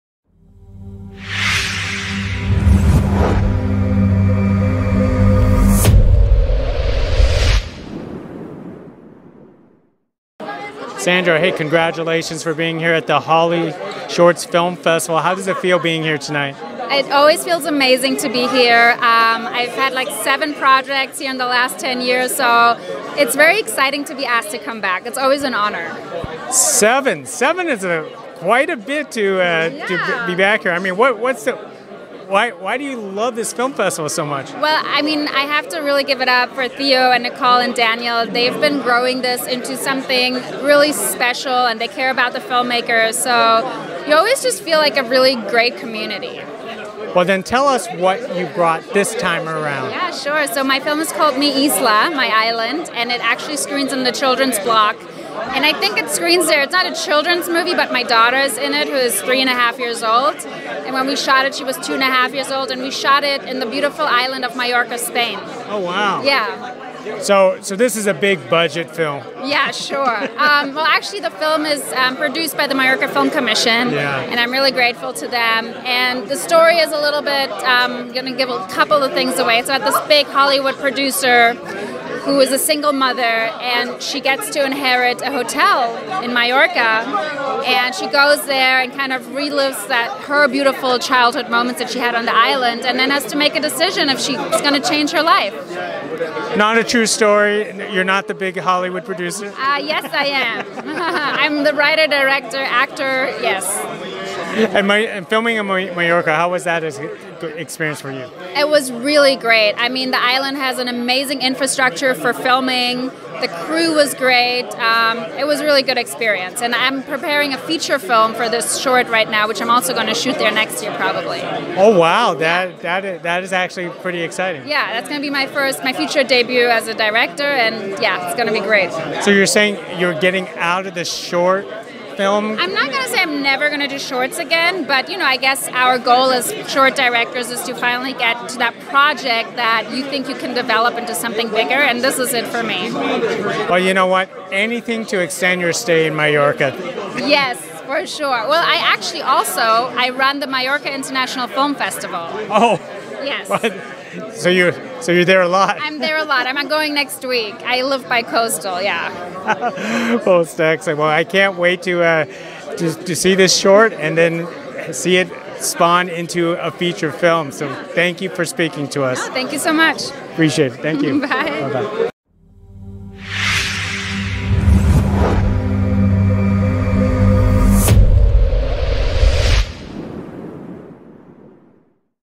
Sandra Lipski Carpet Interview for Mi Isla | HollyShorts Film Festival 2022
LRM Online's Gig Patta spoke with director Sandra Lipski for the short film Mi Isla at the HollyShorts Film Festival 2022.